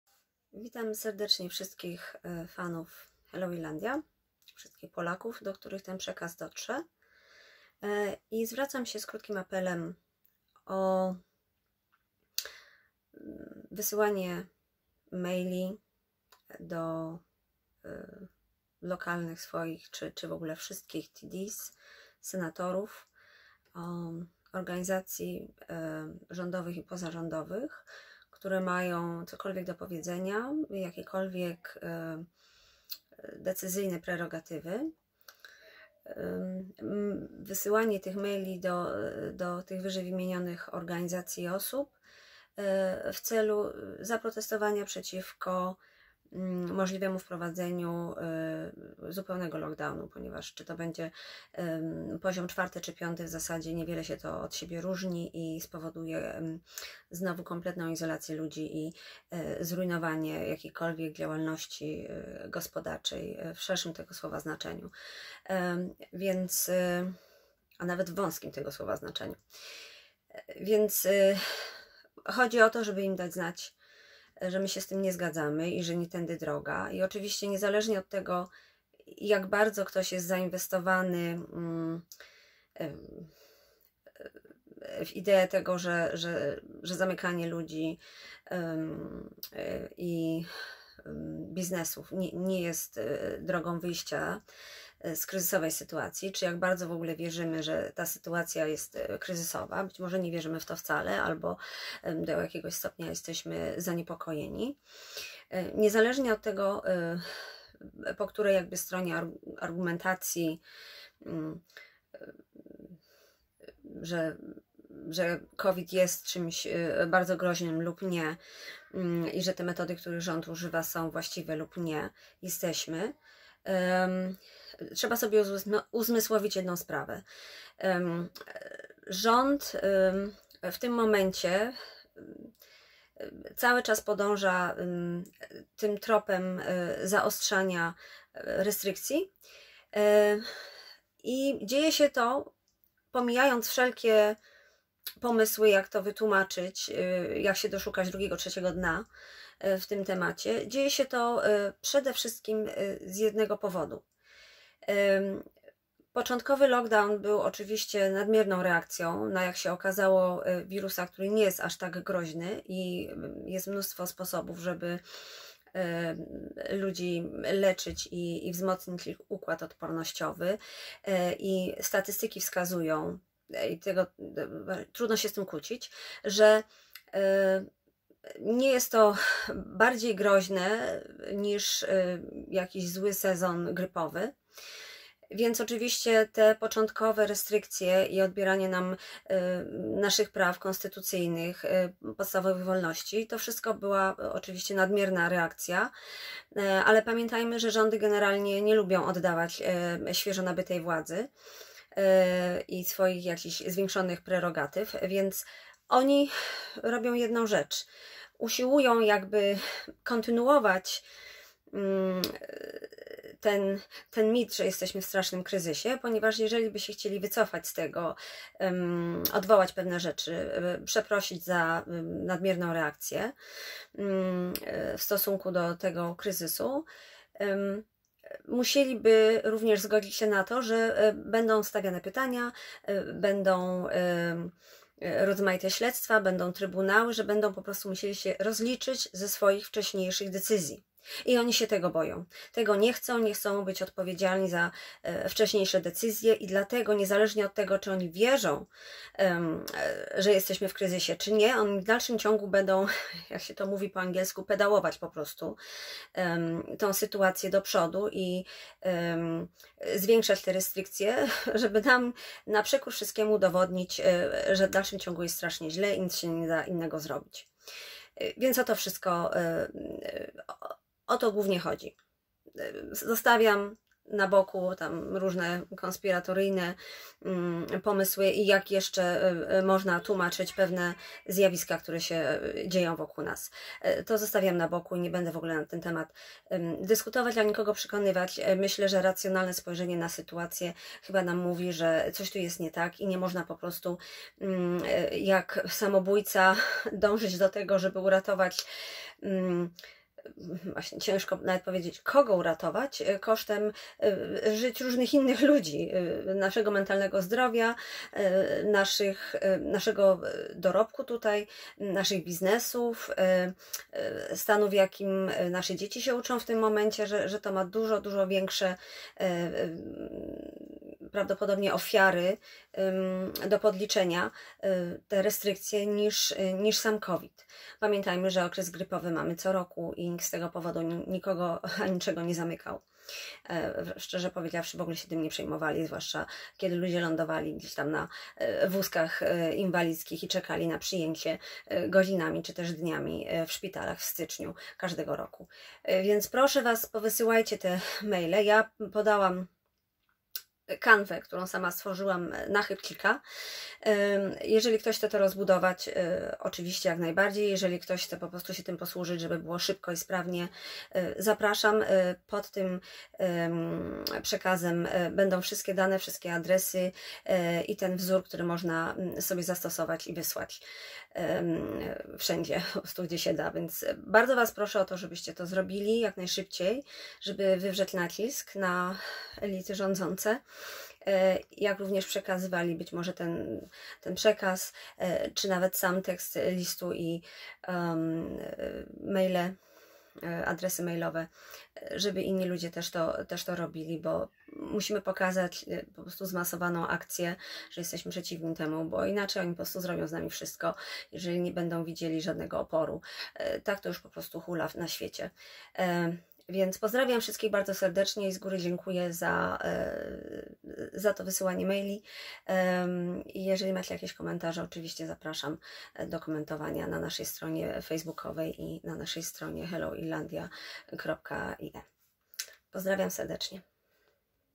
Apel w sprawie Lockdown w Irlandii
Zwracamy się z prośbą o wsparcie apelu o wysyłanie e-maili do deputowanych, senatorów i organizacji rządowych w celu oprotestowania proponowanego ponownego Lockdawn w Irlandii ☘️